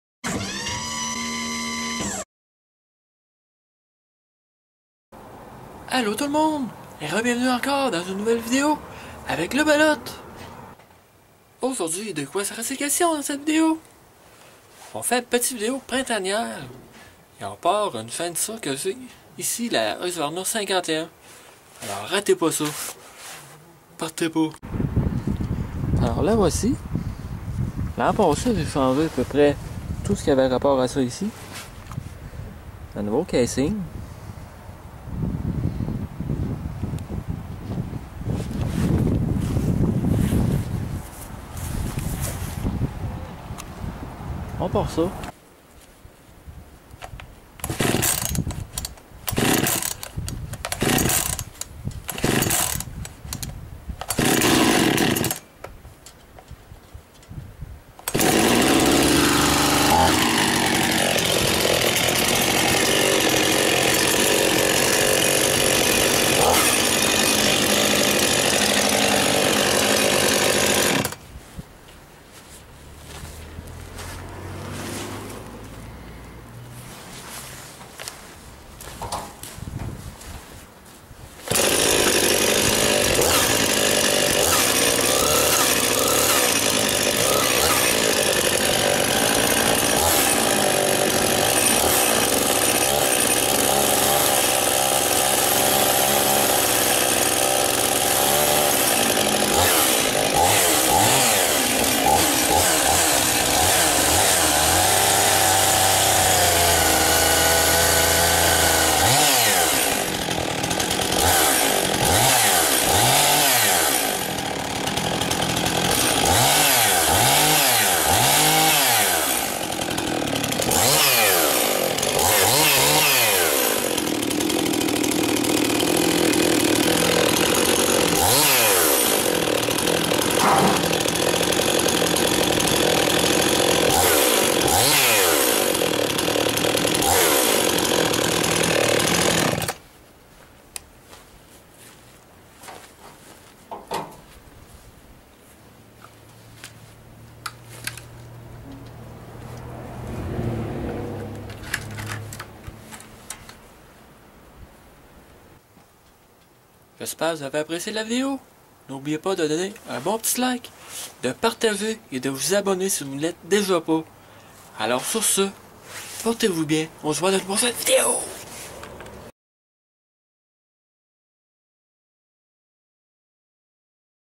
Premier démarrage cette année de la Husqvarna 51
Une belle petite vidéo du premier démarrage de ma Husqvarna 51 cette année